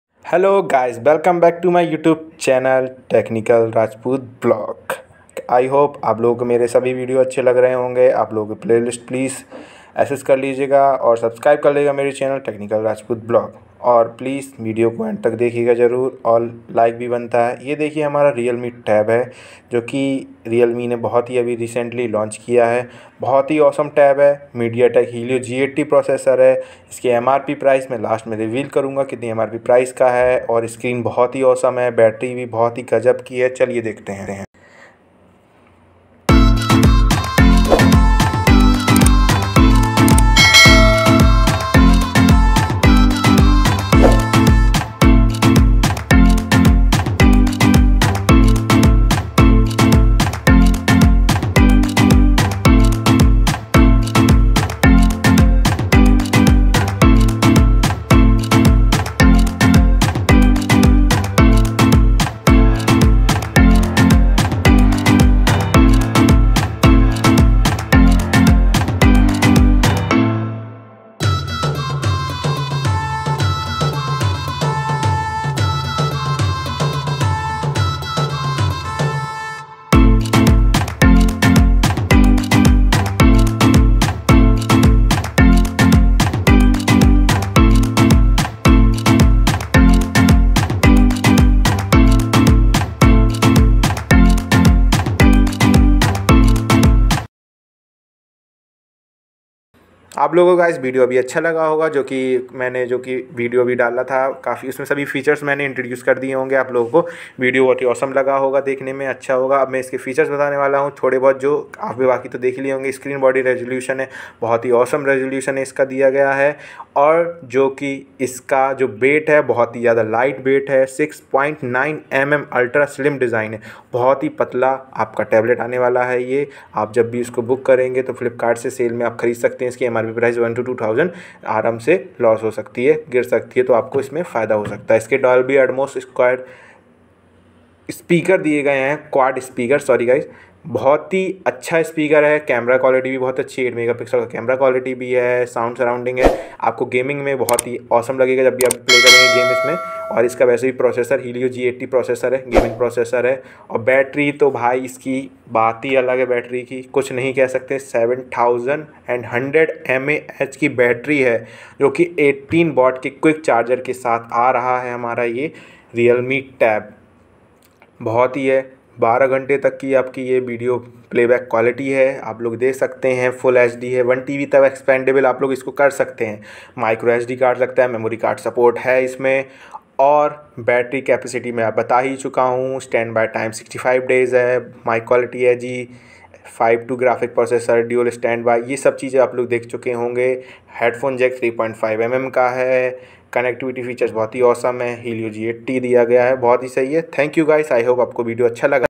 Realme pad Reviews & First impression ⚡⚡| Media tek Helio g80|Big screen 7100 mah battery 🔋 Realme
Hello guys ajj mai apke lye bhut hi awesome video laya hu iss video mai apko Realme tab ke baare mai pura btaoga aur video ko pls guys end tak dekhna
In this video realme tab ke features hai joki Helio g80 processor hai gaming battery 🔋 bhut sahe hai screen pura video mai btauga